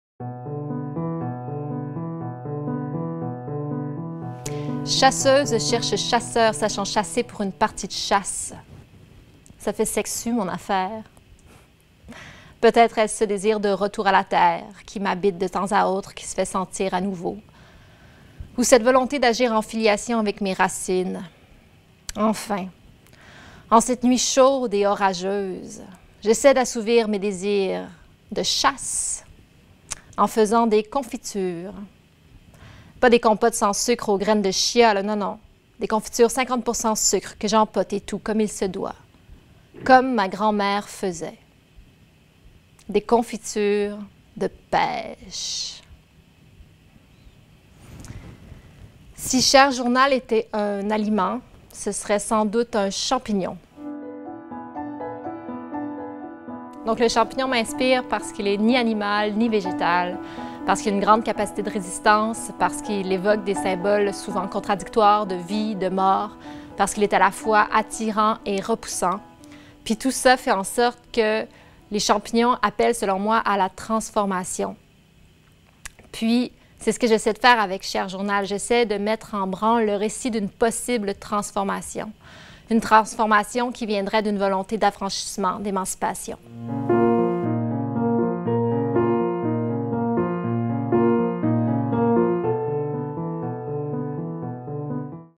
Pénélope Deraîche-Dallaire pour la Salle des machines
Pénélope Deraîche-Dallaire présente Cher Journal, au programme de la Salle des machines le 16 novembre 2022.

Image et montage @emmanuelle boileau